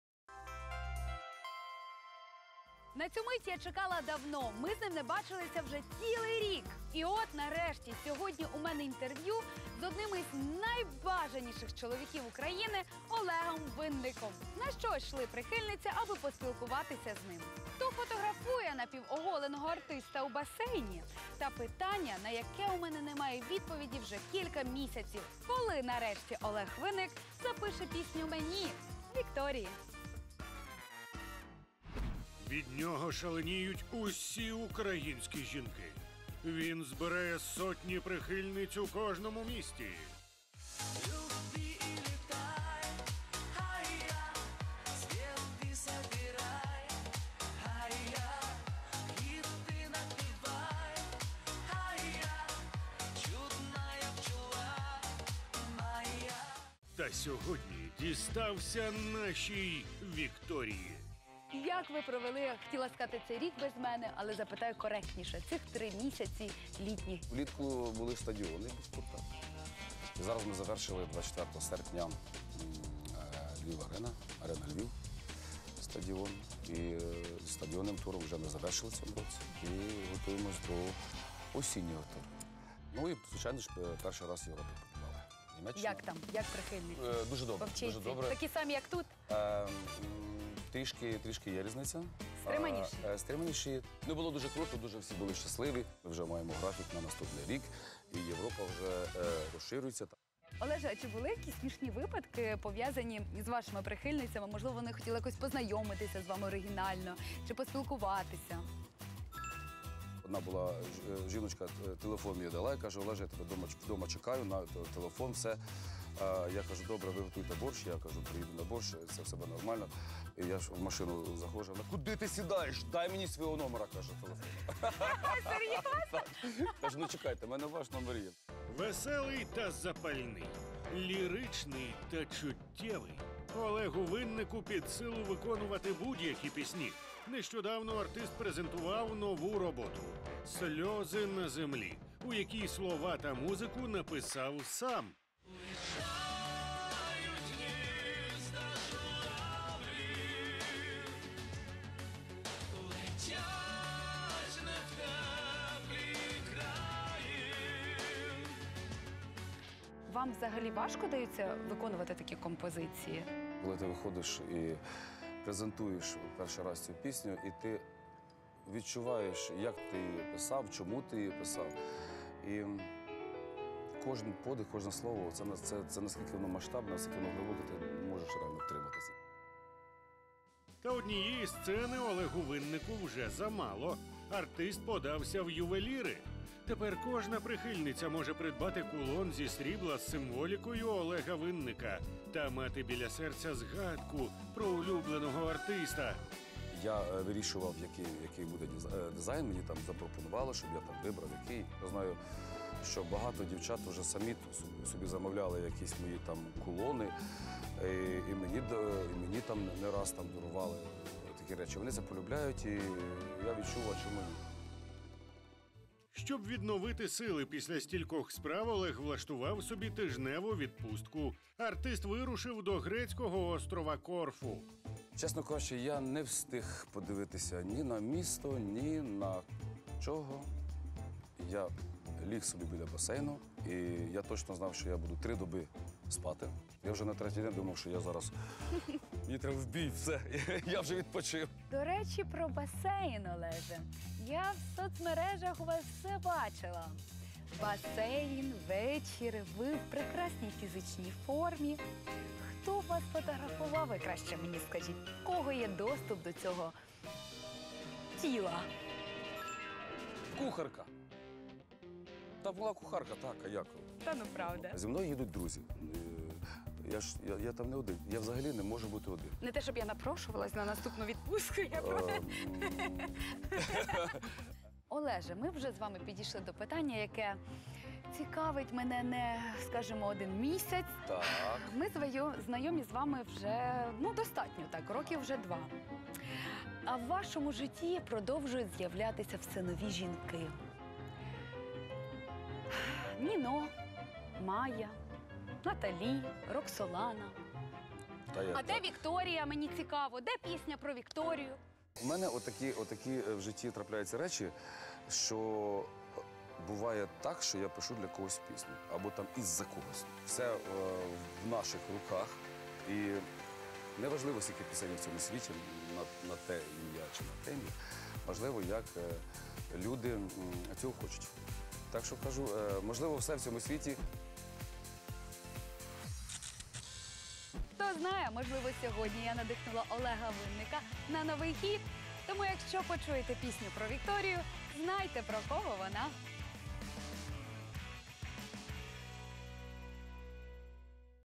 На що йшли прихильниці Олега Винника, аби поспілкуватися з ним | Ранок з Україною
Хто фотографую напівоголеного співака і навіщо? Дізнайтесь далі.